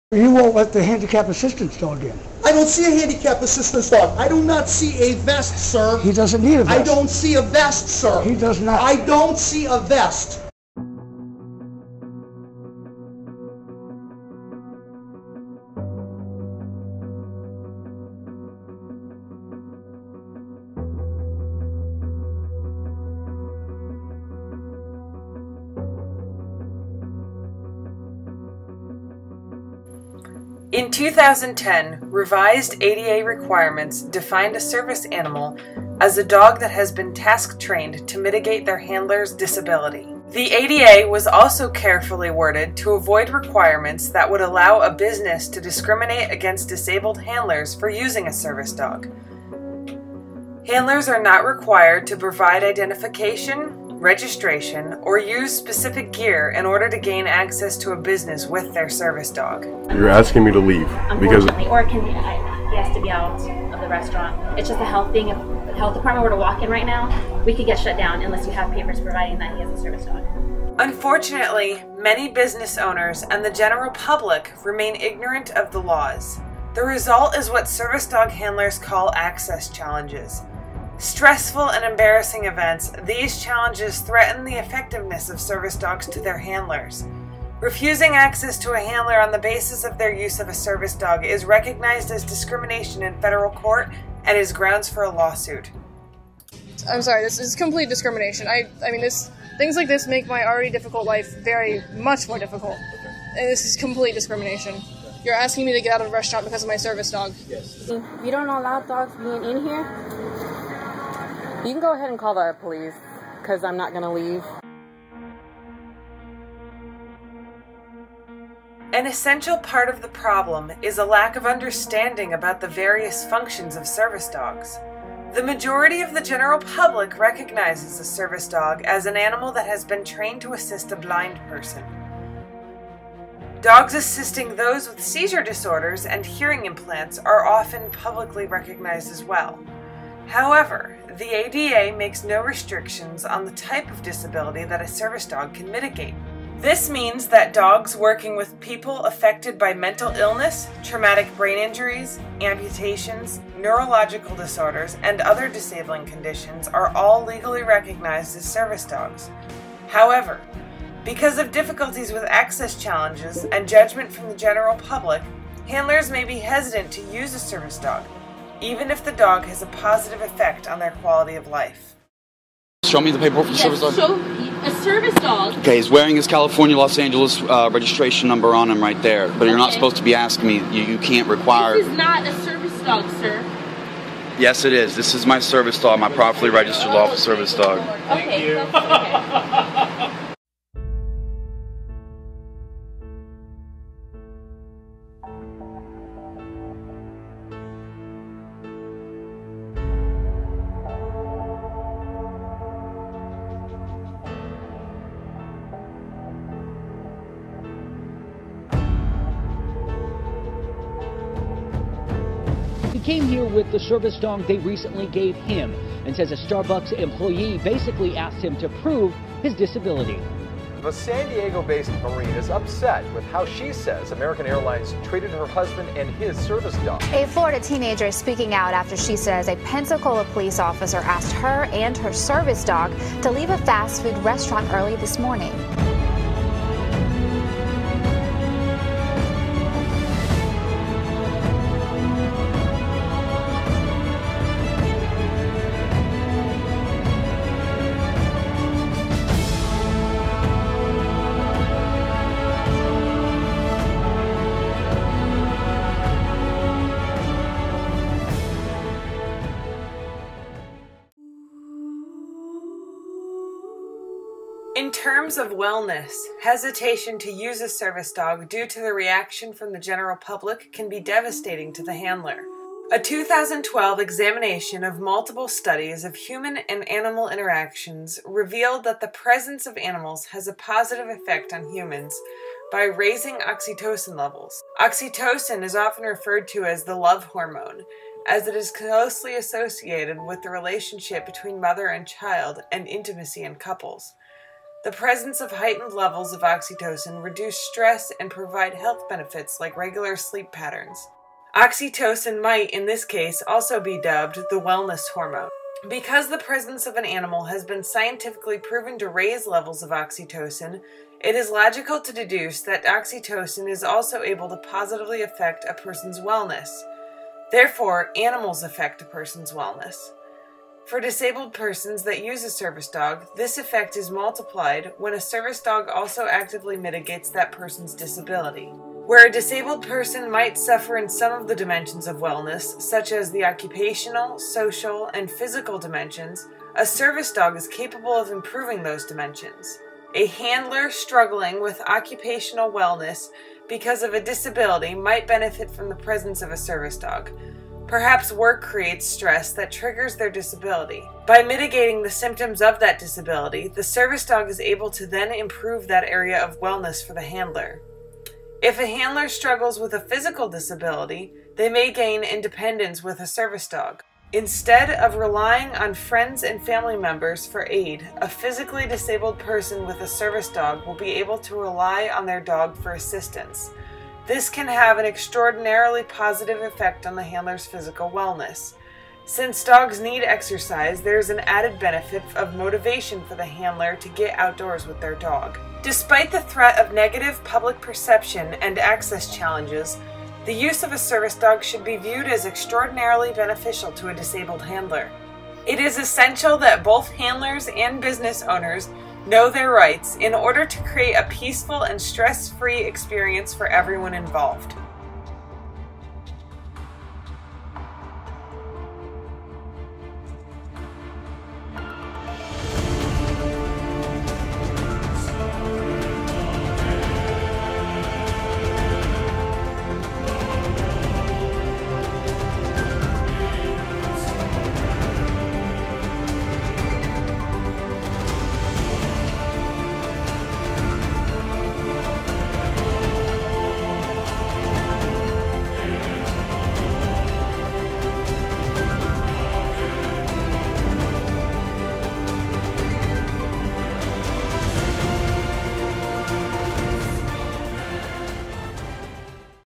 Service Dog Wellness
The important factors that every potential service dog handler considers before getting a service dog include fear of negative public perception and access challenges.